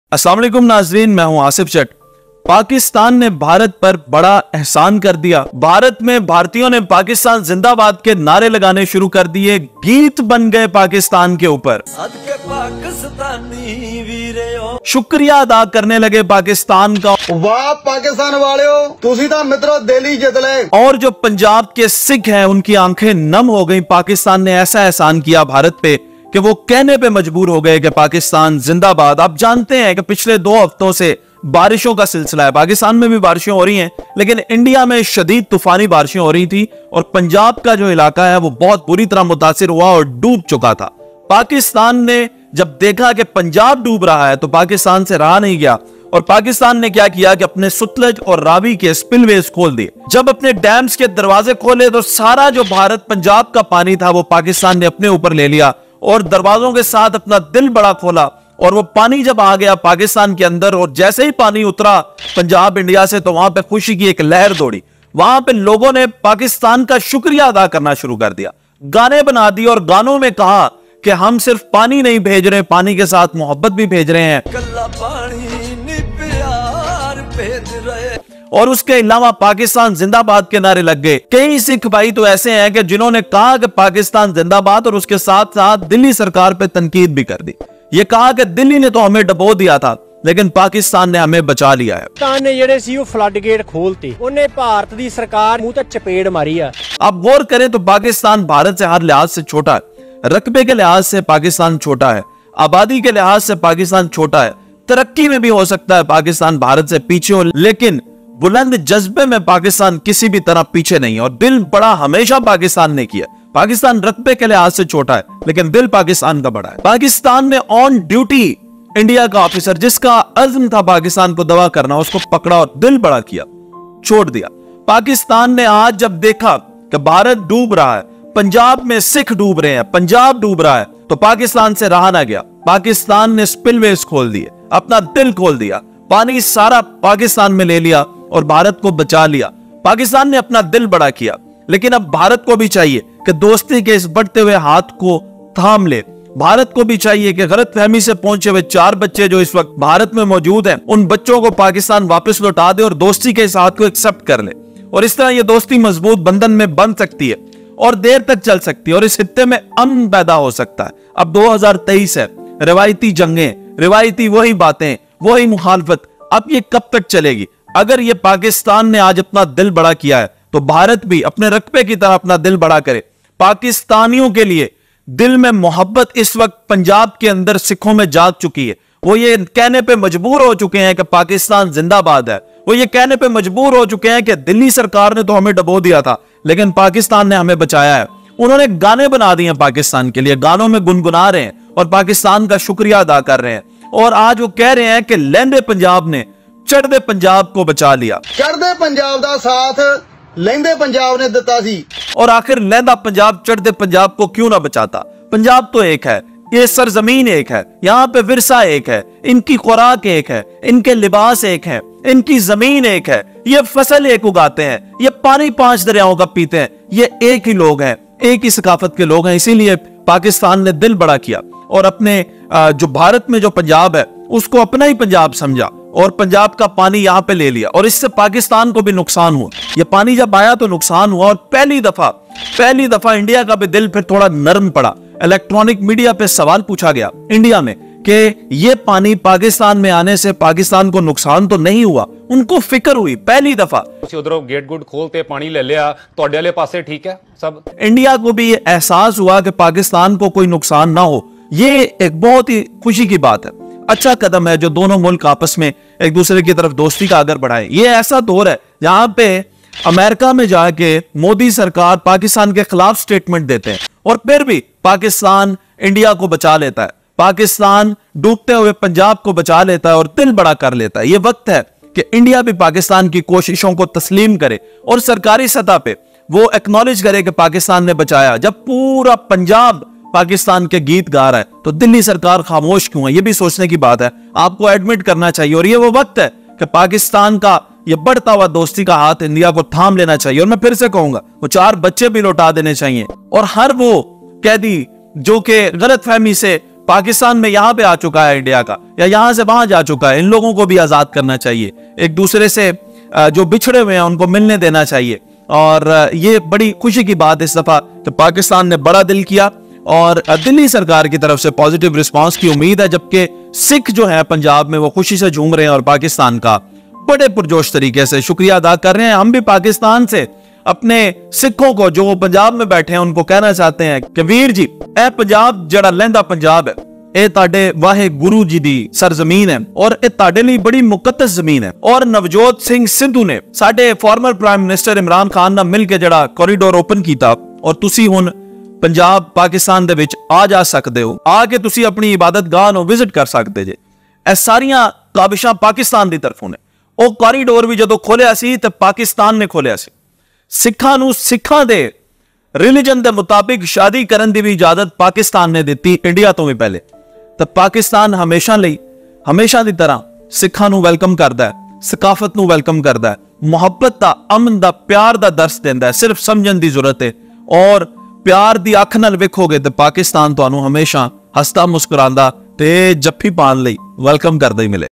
India Mein Pakistan Zinda Baad K Nare Buland - Pakistan Ne India Ko Dobne Se Bacha Liya
India Mein Pakistan Zinda Baad K Nare Buland - Pakistan Ne India Ko Dubane Se Bacha Liya